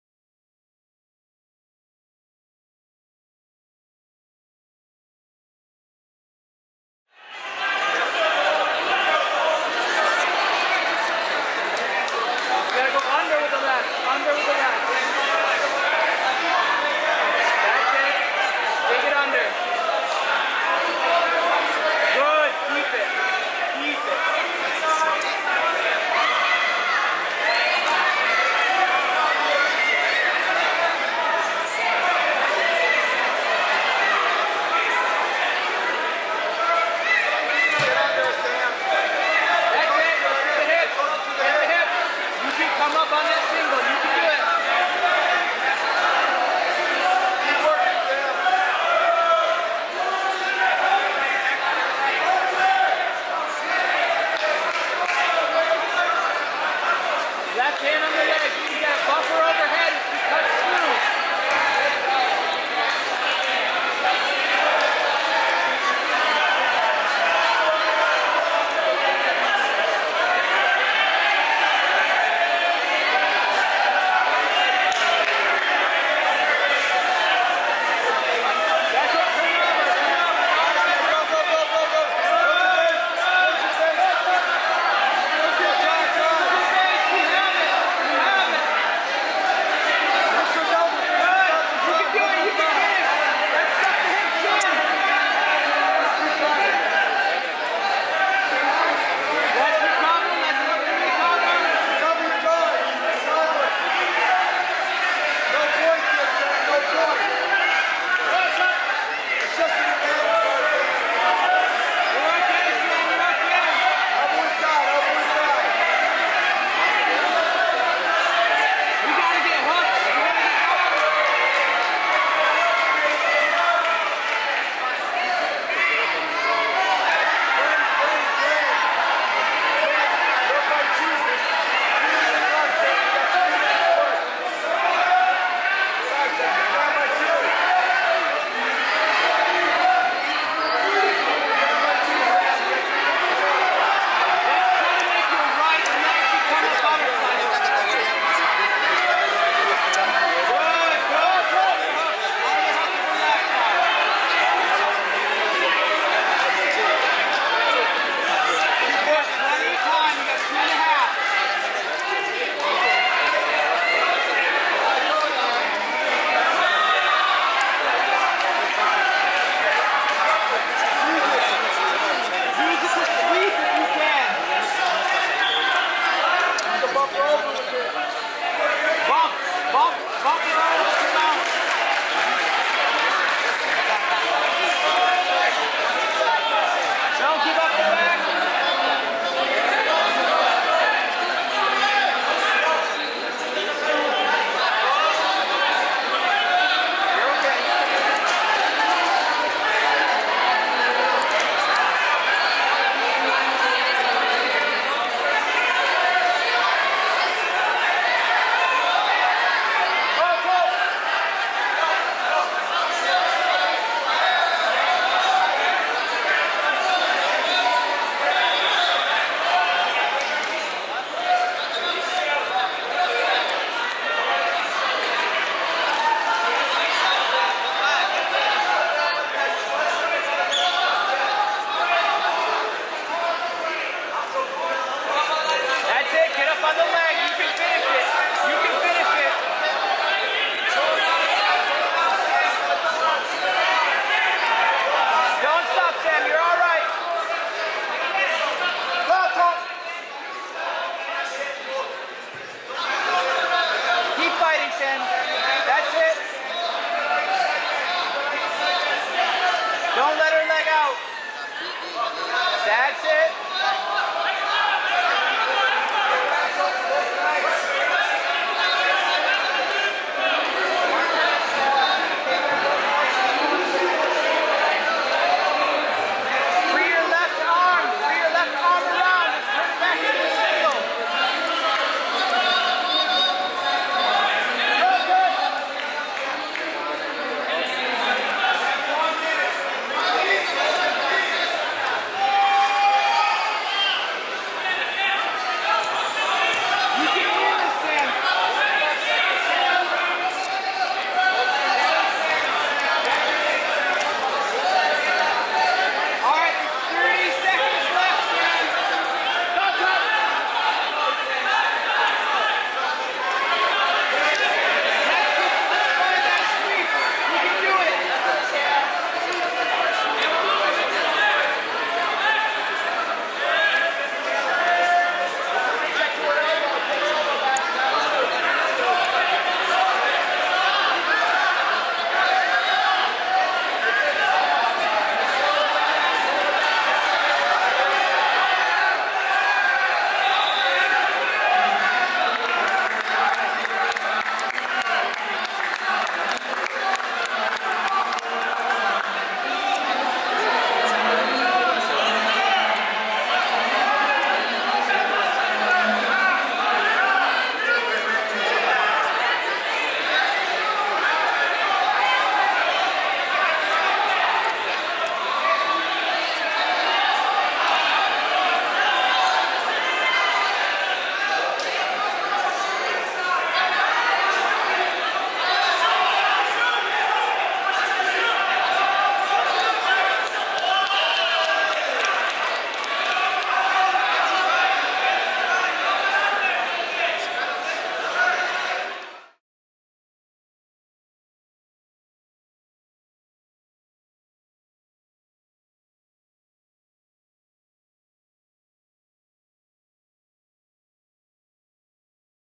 Women's Brazilian Jiu-Jitsu: NY Fall Open Carolina Vallejo Win
Female Brazilian Jiu-Jitsu match at the 2019 NY Fall Open.

Internationally, less than 4% of sports coverage showcases female athletes.